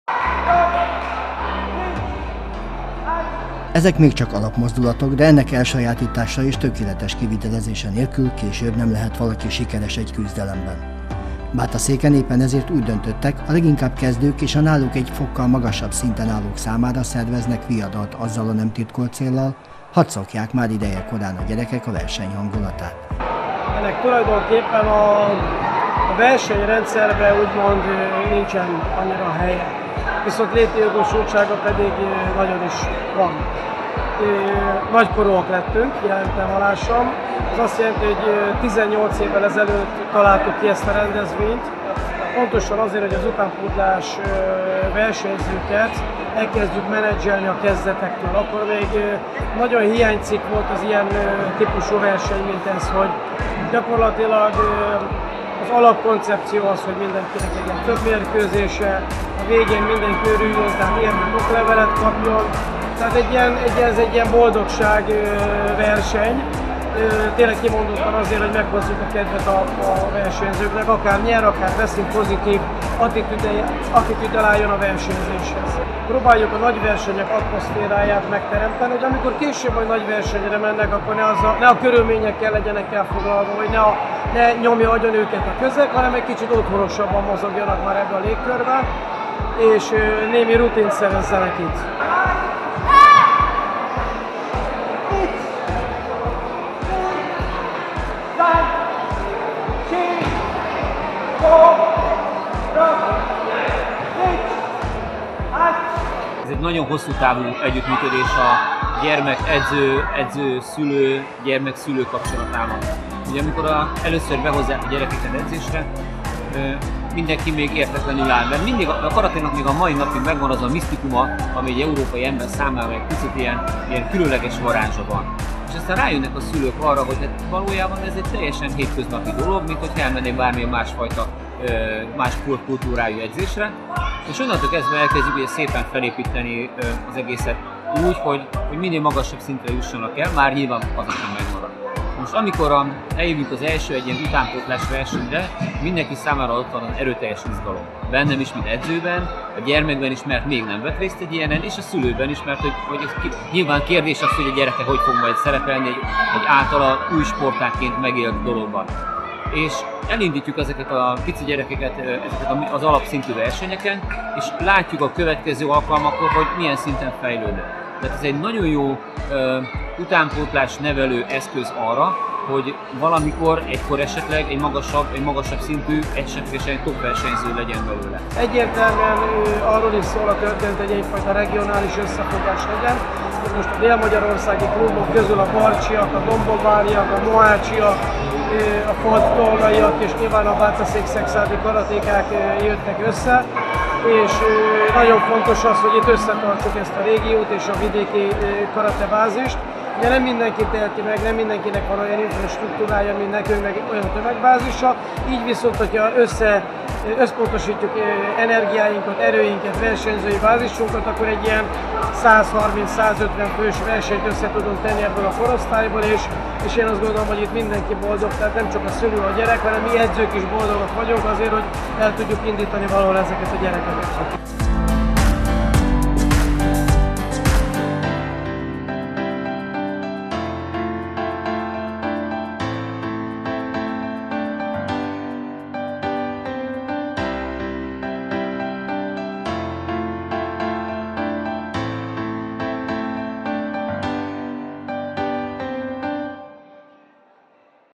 Közel 150 utánpótlás korú karatéka mérte össze tudását Bátaszéken
Közel 150 utánpótlás korú karatéka mérte össze tudását a bátaszéki sportcsarnokban. A versenyre a régió egyesületei hozták el azokat a versenyzőket akik most tanulnak bele a sportág mesterfogásaiba. A viadal célja az hogy idejekorán hozzá szokjanak ahhoz a közeghez, amelyet egy versenyhelyzetben át kell élni